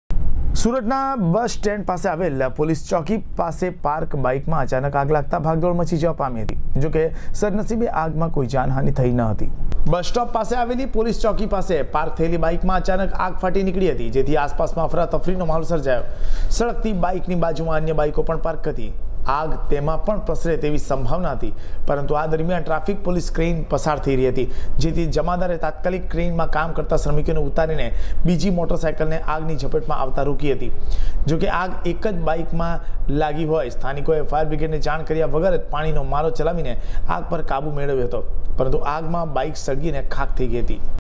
bike fire સુરતના બસ સ્ટેન્ડ પાસે આવેલ પોલીસ ચોકી પાસે પાર્ક બઈકમાં અચાનક આગ
લાગતા ભાગદોડ મચી જવા પામી હતી. જો કે સહદનીસે આગમાં કોઈ જાનહાની થઈ ન
હતી.